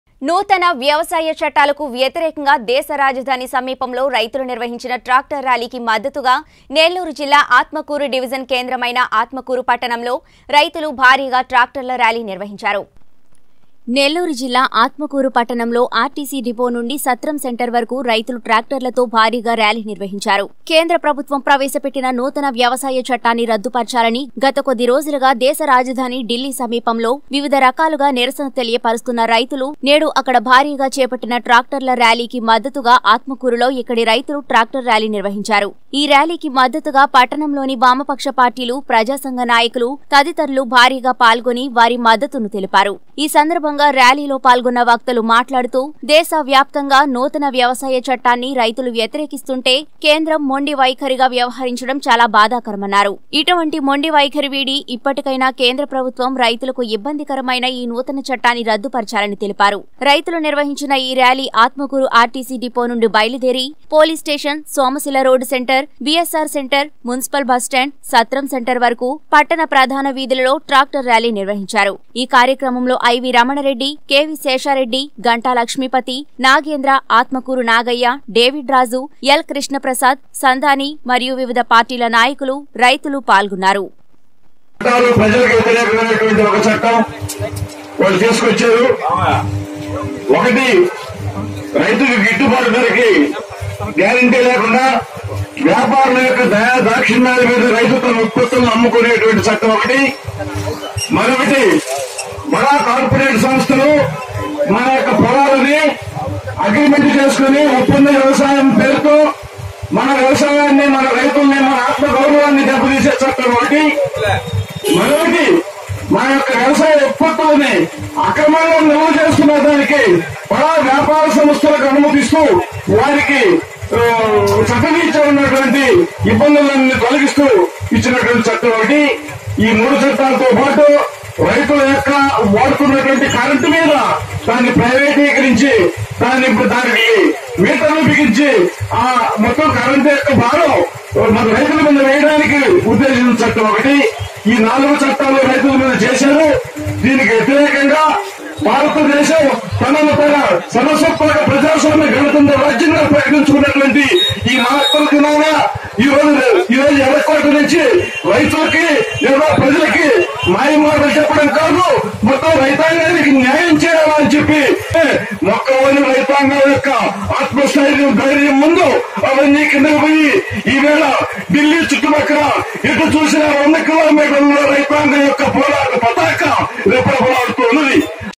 నెల్లూరు జిల్లా ఆత్మకూరులో నూతన వ్యవసాయ చట్టాలకు వ్యతిరేకంగా రైతులు ట్రాక్టర్ల ర్యాలీ
నెల్లూరు జిల్లా ఆత్మకూరులో నూతన వ్యవసాయ చట్టాలకు వ్యతిరేకంగా రైతులు ట్రాక్టర్ల ర్యాలీ | 19 Newstelugu